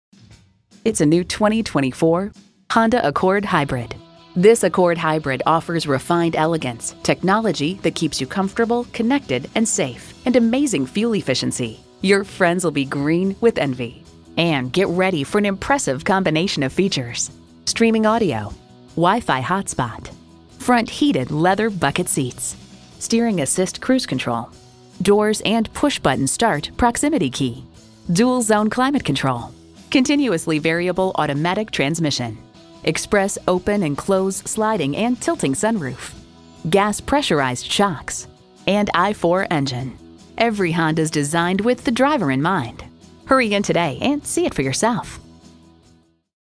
New 2024 Honda Accord West Palm Beach Juno, FL RA072083 - SOLD
Year: 2024
Make: Honda
Model: Accord
Trim: Hybrid EX-L
Transmission: eCVT
Color: Crystal Black Pearl
Interior: Bk/Black
Stock #: RA072083
VIN: 1HGCY2F66RA072083

Address:
3790 West Blue Heron Boulevard